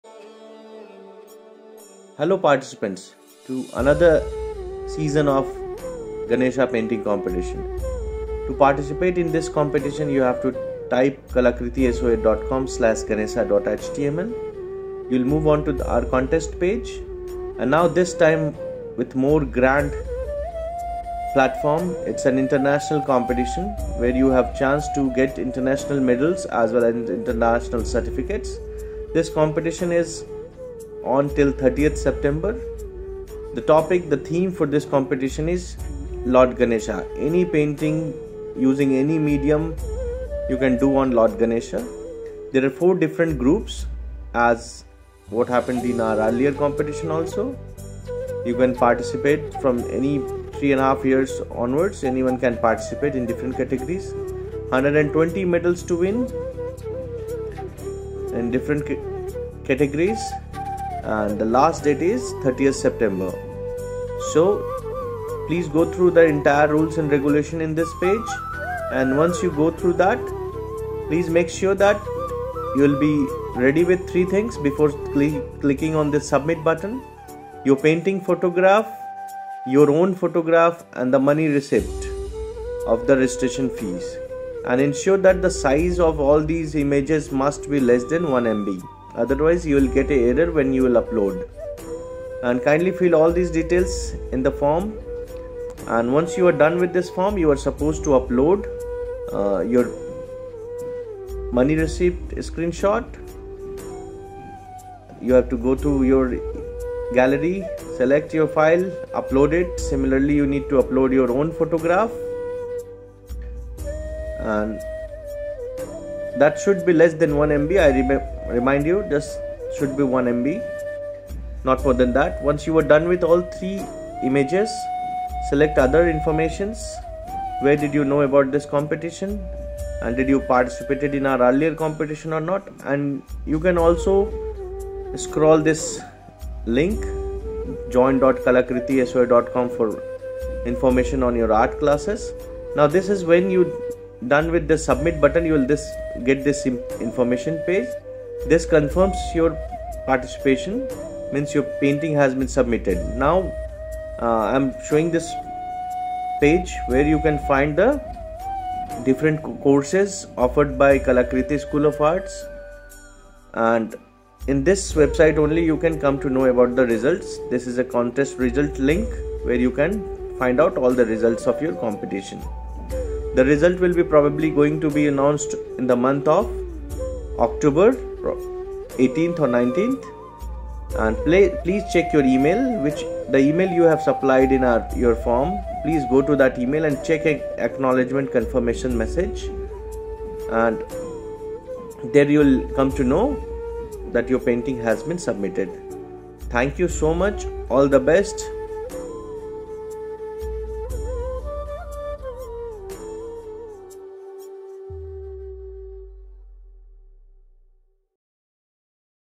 Know How to Submit your Painting in GANESHA Season - 2 International Painting Competition 2021 (Eng)
Announcing GANESHA - Season 2👩🏻‍🎨🎨👨🏼‍🎨

International Painting Competition 2021 organized by Kalakriti Art Foundation, (NGO - Registered Under Society act 1860) Jharkhand and MSME, Govt of India and Intellectual Property Rights, Govt of India

👉  ► 120 International Medals🥇🥈🥉  to WIN

👉  ► Contest Period : 6th Sep to 30th Sep 2021

👉  ► Theme : Lord GANESHA

👉 ► Age Group : Children (3 years above ), Students and Professionals (4 Different Category)